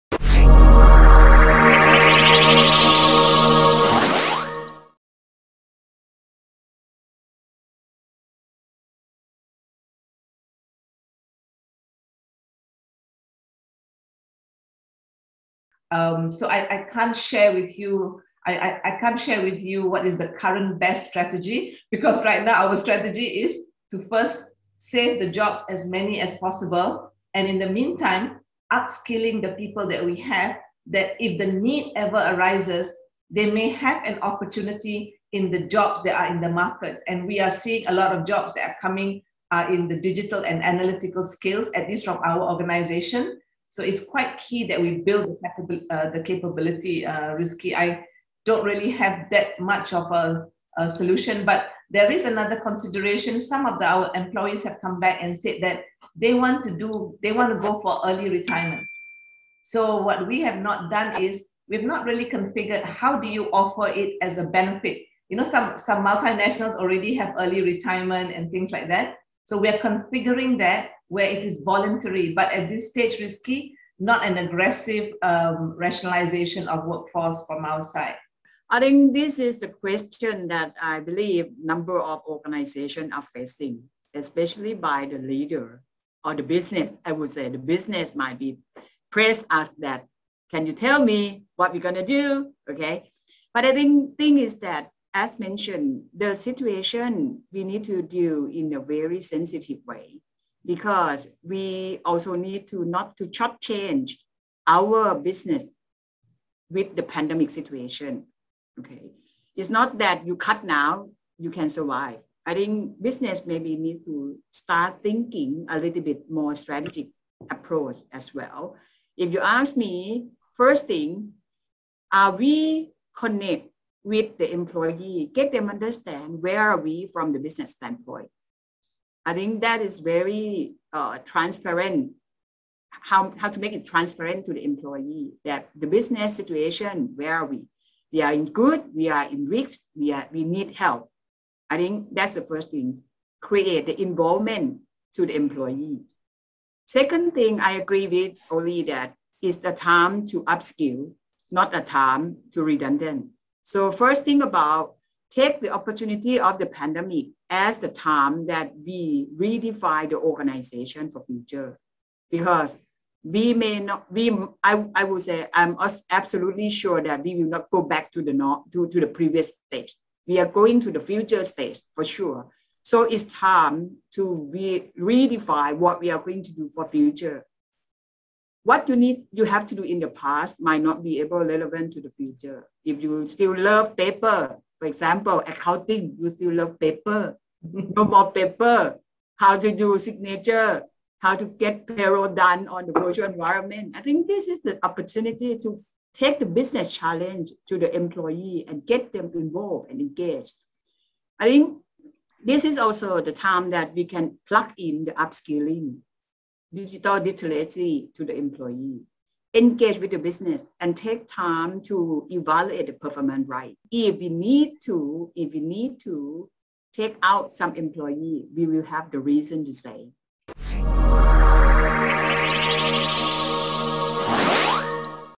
BENARKAH OFFBOARDING DI MASA KRISIS MERUPAKAN ALTERNATIF TERBAIK ? NORLIDA AZMI
Pandemi yang telah berlangsung lebih dari dua tahun telah membawa banyak kesulitan bagi sebagian besar organisasi. Beberapa diantaranya merestrukturisasi  organisasi menjadi lebih ramping, Hal ini  tentunya membuat banyak karyawan  melakukan offboarding. Namun benarkah offbording merupakan alternatif terbaik ketika menghadapi kesulitan di masa pandemi ? Norlida Azmi dari Axiata Malaysia memberikan pendapatnya dalam video ini.